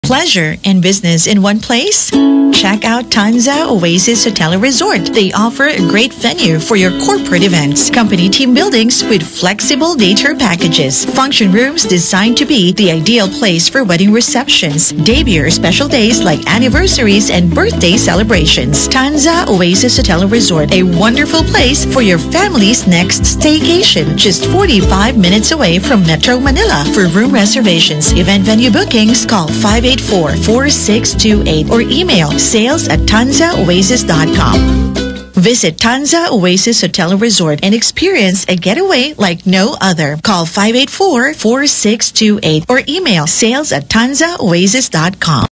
Tanza Oasis Hotel and Resort Radio Ad (2019) (Voiced by DJ Chloe of 96.3 Easy Rock)
Visit Tanza Oasis Hotel and Resort and get away like no other